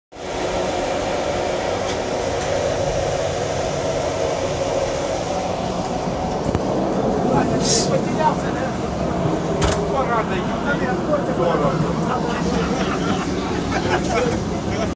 "Якість від УЗ туалет ер9е-62901 Регіональний поїзд №818 Київ-Ворожба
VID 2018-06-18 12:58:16

Ни крана,ни бумаги,окно не закрашено.
"качество УЗ туалет эр9е-62901 Региональный поезд №818 Киев-Ворожба